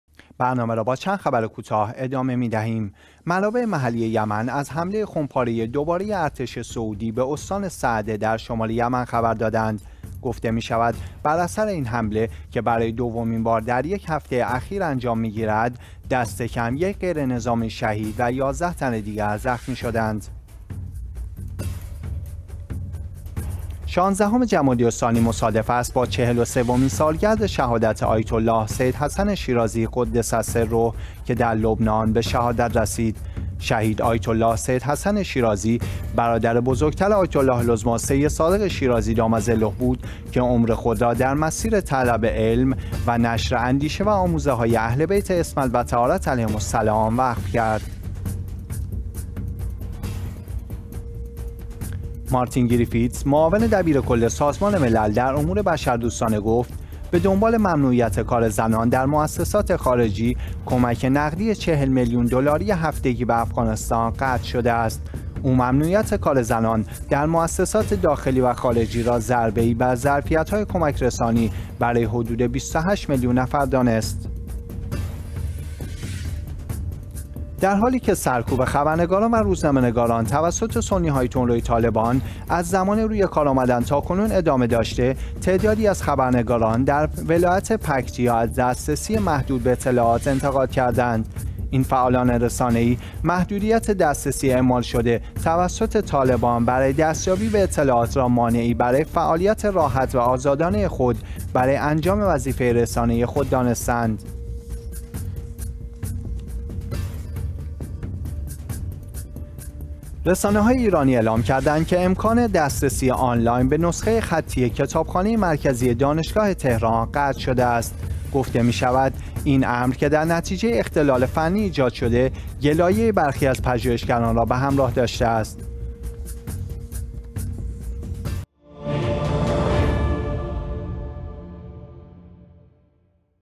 نگاهی به چند خبر کوتاه: يكشنبه 18 ديماه 1401
۱)حمله دوباره ارتش سعودی به شمال یمن
2)برگزاری چهل و سومین سالگرد شهادت آیت الله سید حسن شیرازی
3)سازمان ملل کمک‌های نقدی به افغانستان را قطع کرد
4)انتقاد خبرنگاران افغانستان از دسترسی محدود به اطلاعات
5)حذف نسخه های خطی از سایت کتابخانه مرکزی دانشگاه تهران